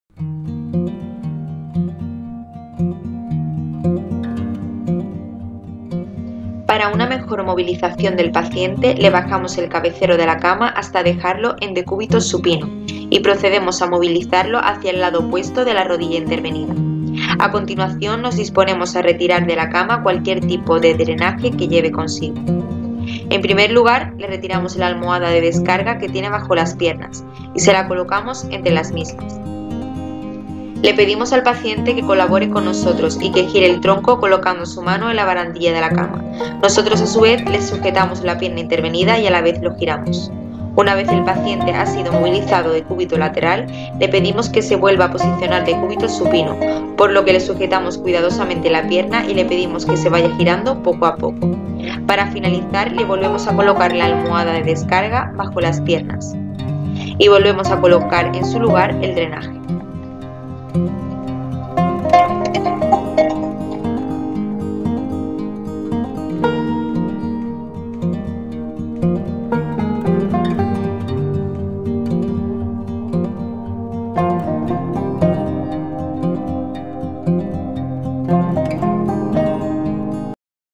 Movilización de pacientes intervenidos con prótesis de rodilla.
En este vídeo profesionales de enfermería de la planta de hospitalización quirúrgica nos explican la técnica adecuada para movilizar a un paciente intervenido de prótesis de rodilla.